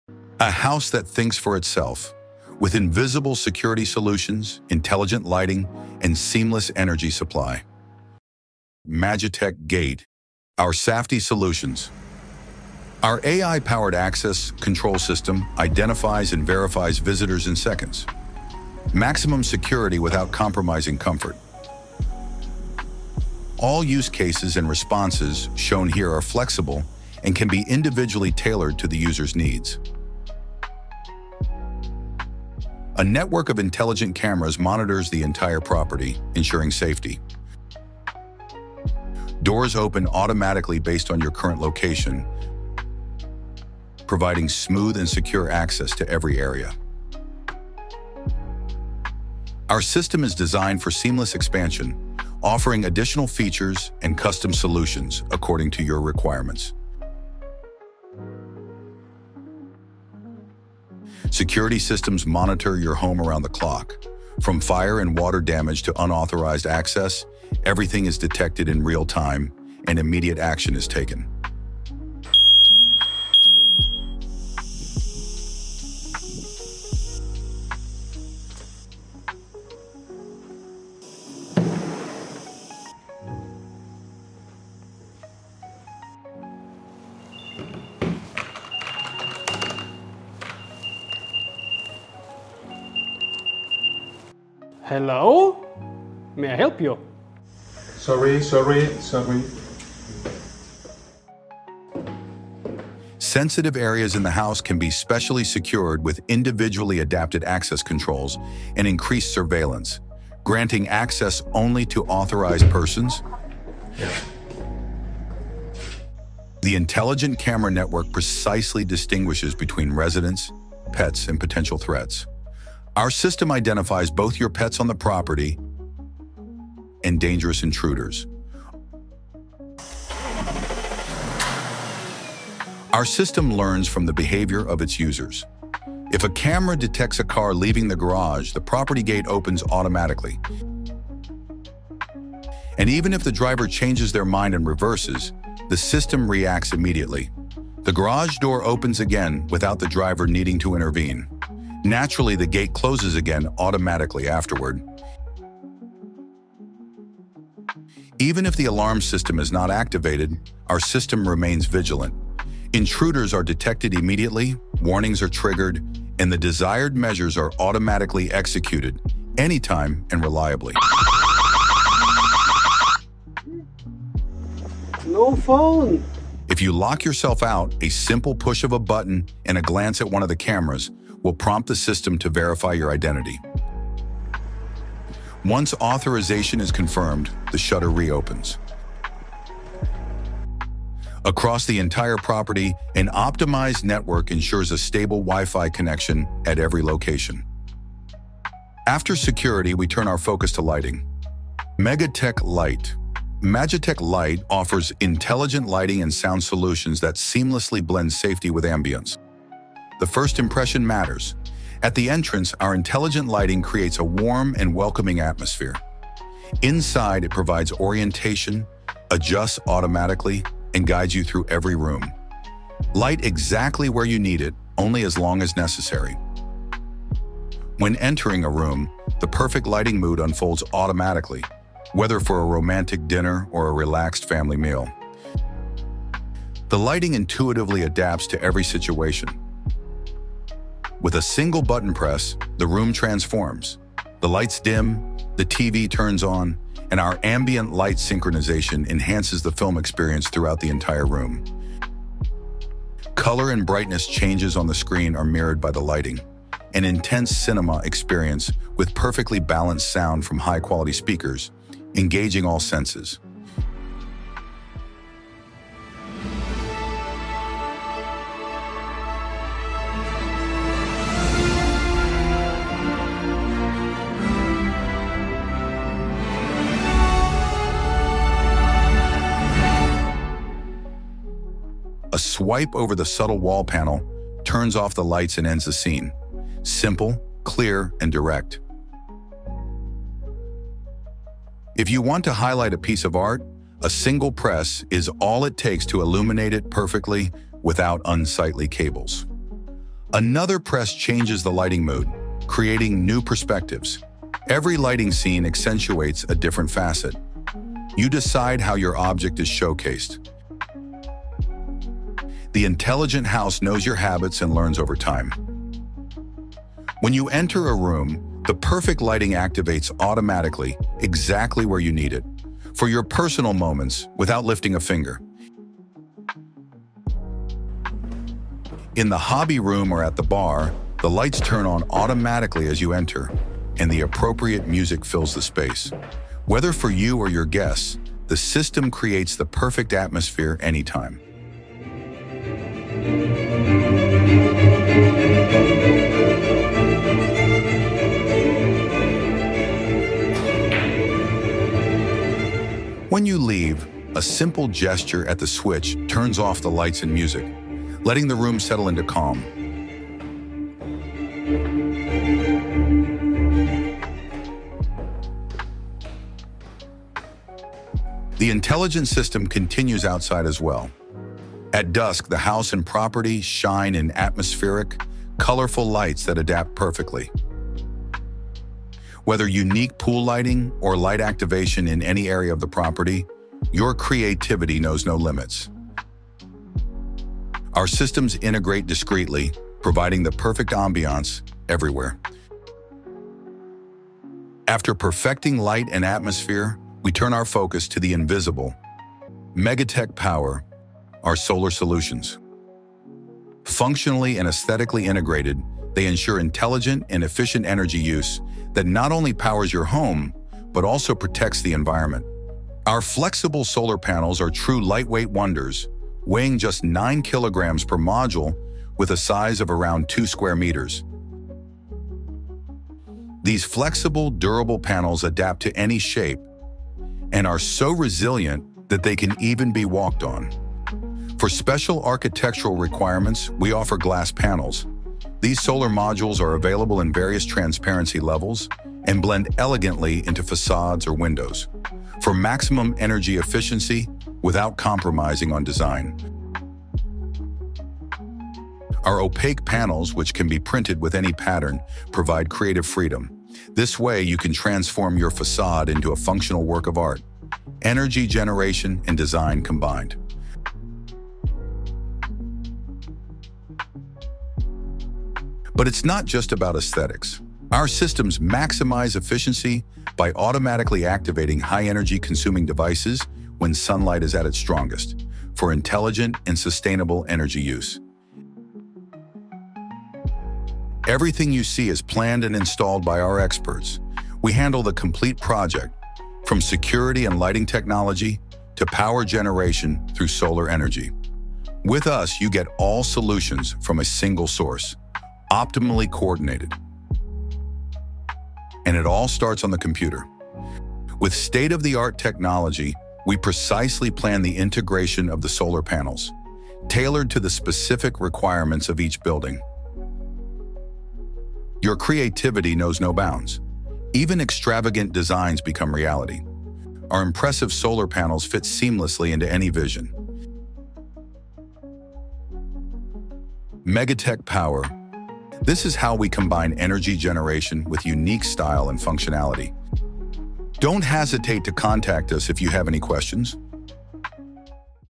The Most Advanced AI Home Setup | Control Everything with AI | Complete AI Home Setup Explained
The Most Advanced AI Home Setup | Control Everything with AI | Smart Home Explained

Welcome to the world of Magitech – the ultimate AI Smart Home experience! – The AI-powered Smart Home. Magitech brings cutting-edge technology to every corner of your home, from intelligent lighting and energy-efficient solar designs to innovative access systems. Experience how artificial intelligence and thoughtful design make your home more comfortable, secure, and sustainable. Magitech – for a smart home that adapts to your needs.

What You’ll Learn:

How AI controls every corner of your home
Smart automation features for comfort, security, and energy efficiency
A glimpse of futuristic home technology in real life
Why Magitech is leading the smart home revolution